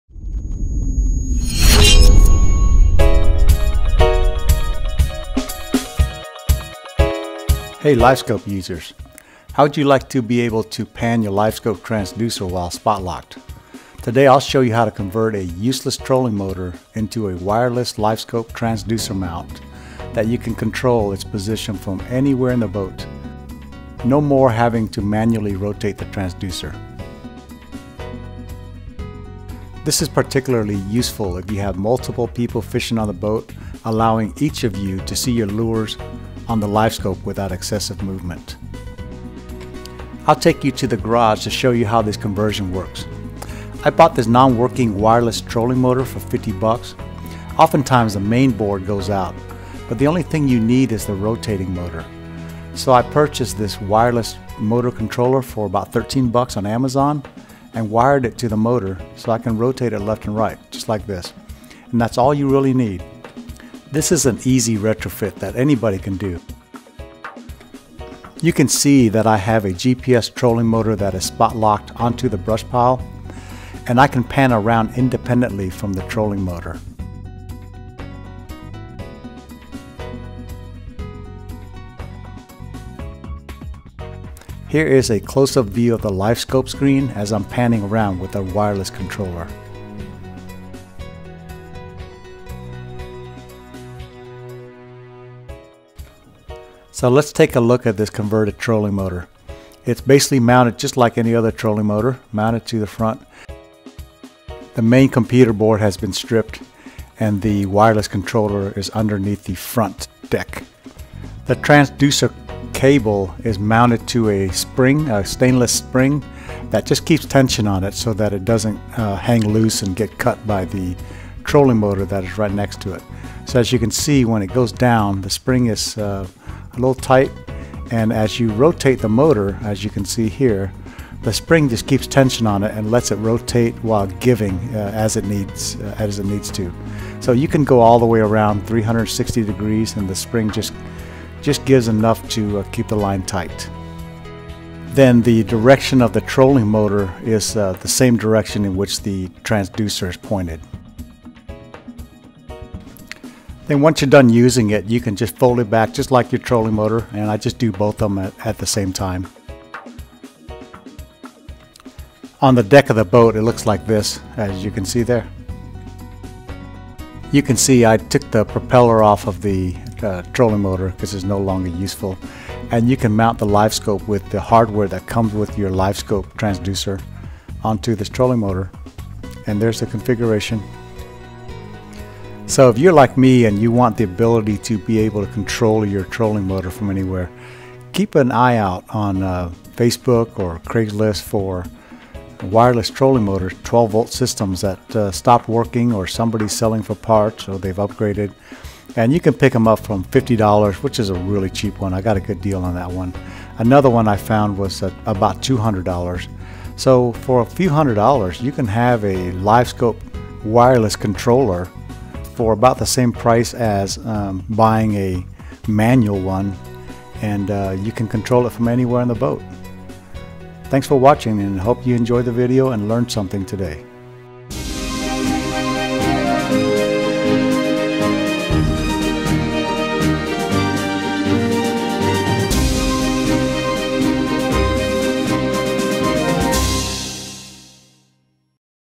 How to DIY Wireless Control Garmin Livescope Transducer mount
Hey Livescope users, how would you like to be able to pan your livescope transducer while spot locked?

I'll show you how to convert a useless trolling motor into the wireless LVS transducer mount that you can control it's position from anywhere in the boat.  No more having to manually rotate the transducer.  I hope this helps you.  Enjoy the video.
Tim

Wireless Motor Controller